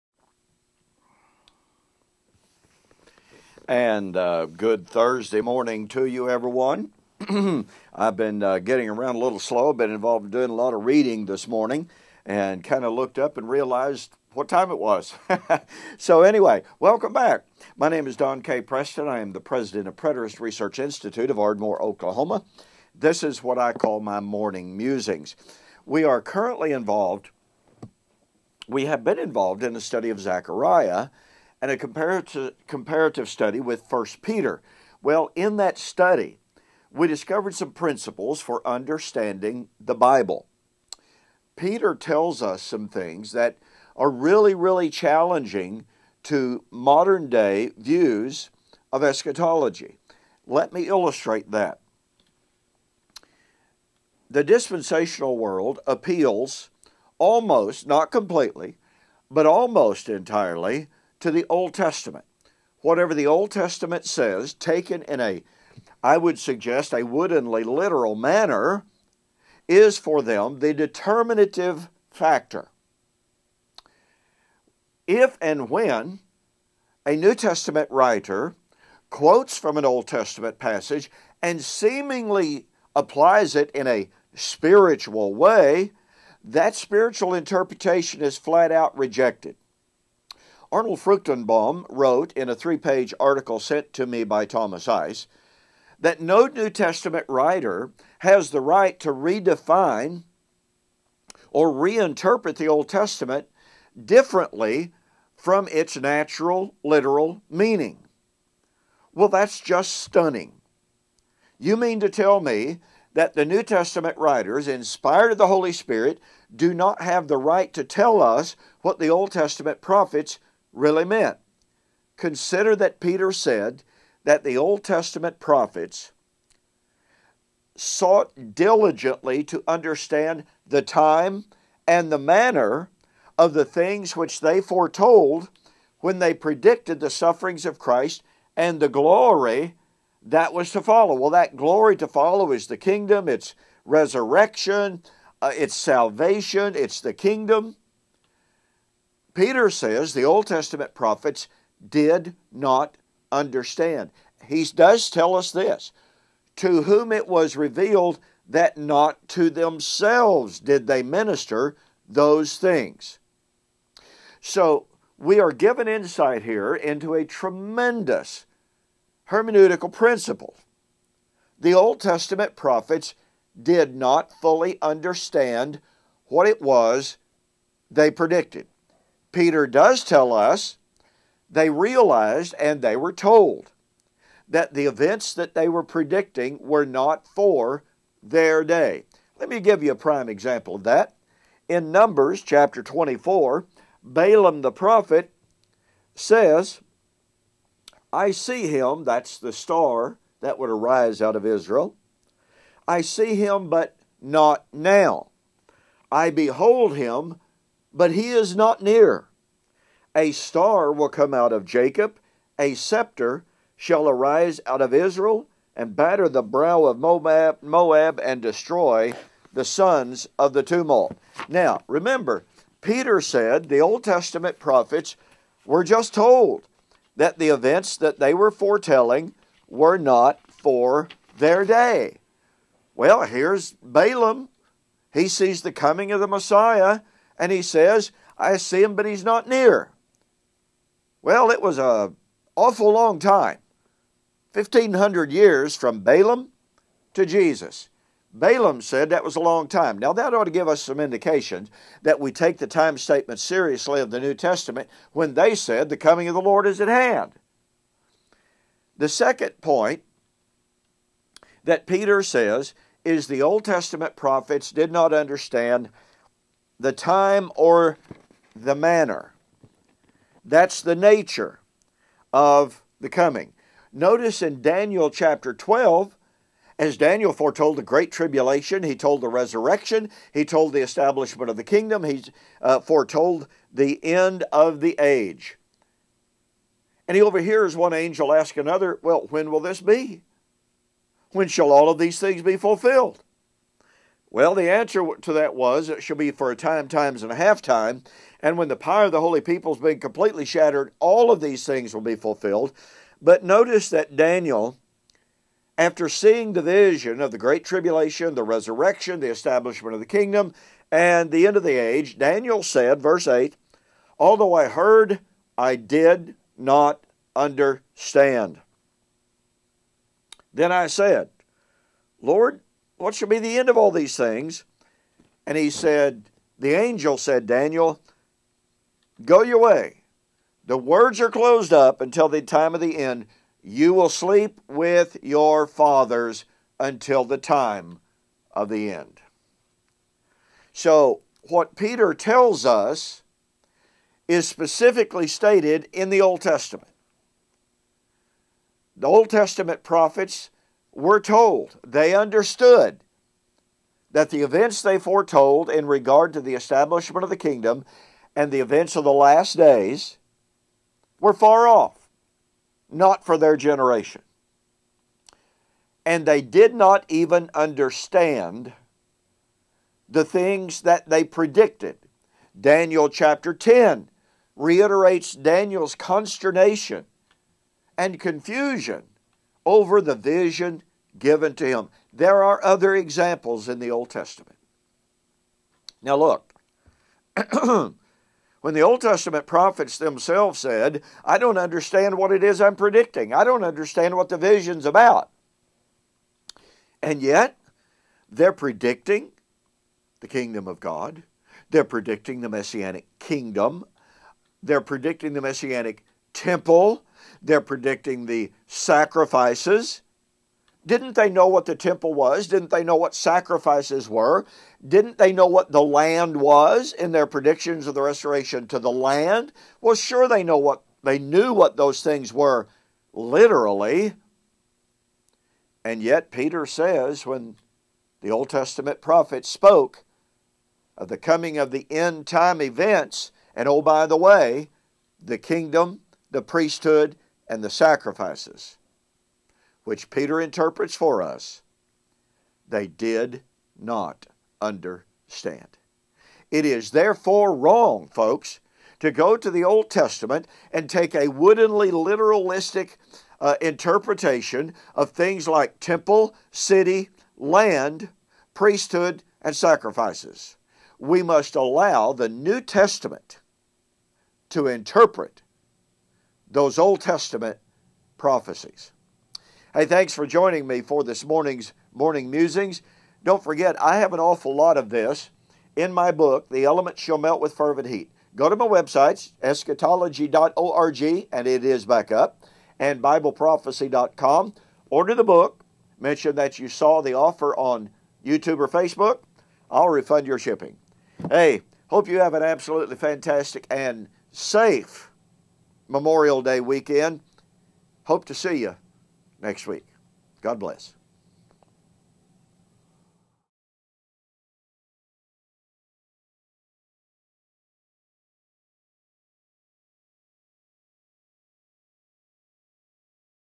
Principles for Understanding Bible Prophecy #3
Peter is emphatic that the OT prophets did not know the time or the manner of the end time things they foretold. This has profound implications for our understanding of the OT-- and the NT interpretation of those OT prophecies.